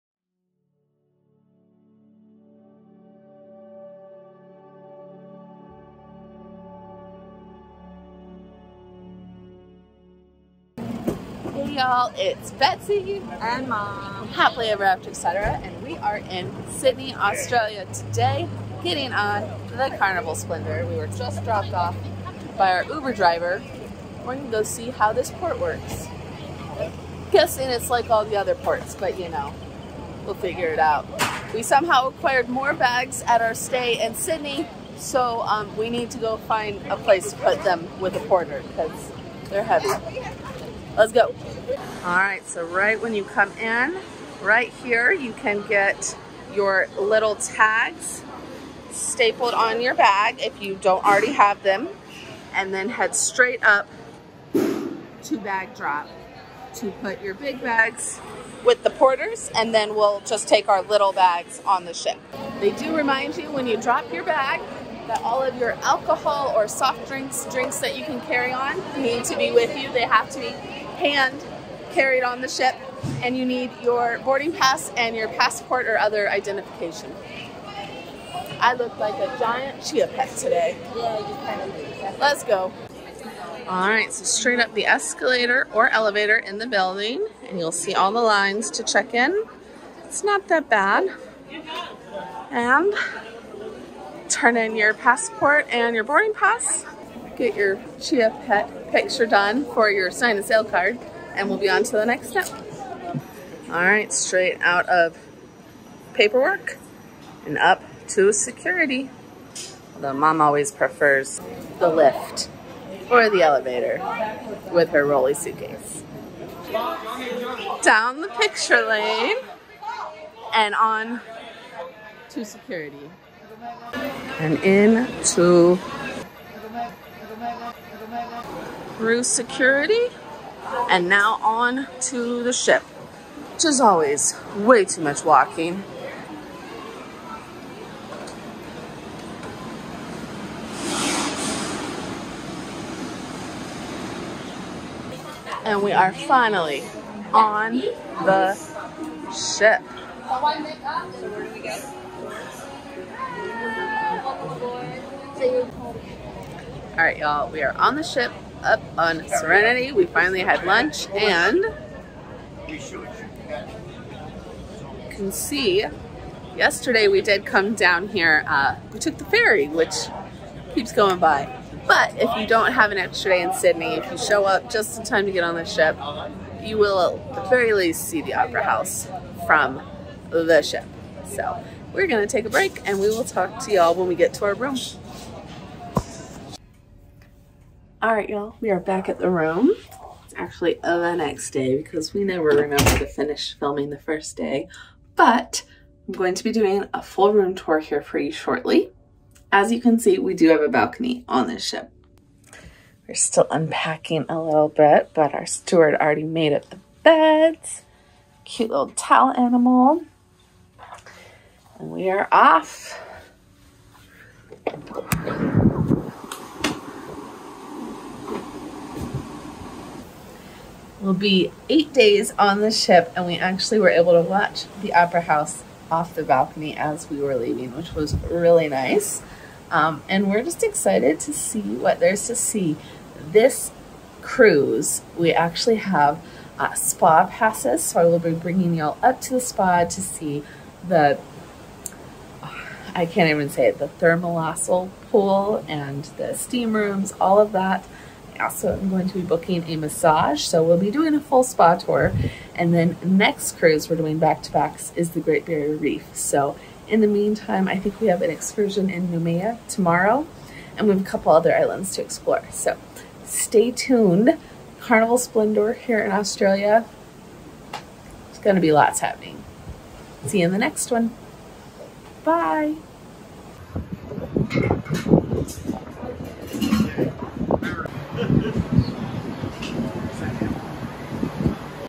Boarding the Carnival Splendor... continuing our Australian Cruise Adventure!
Today we are finally boarding the Carnival Splendor! This ship is currently docked directly across from the Sydney Opera House so I'm DOUBLE excited for embarkation day! Let's get on board and explore!

11 Day Cruise Itinerary on the Carnival Luminosa:
Day 1: Embarkation Day
Day 2: Day at Sea
Day 3: Day at Sea
Day 4: Santo, Vanuatu
Day 5: Port Villa, Vanuatu
Day 6: Day at Sea
Day 7: Port Denarau, Fiji
Day 8: Dravuni Island, Fiji
Day 9: Day at Sea
Day 10: Nouméa, New Caledonia
Day 11: Day at Sea
Day 12: Brisbane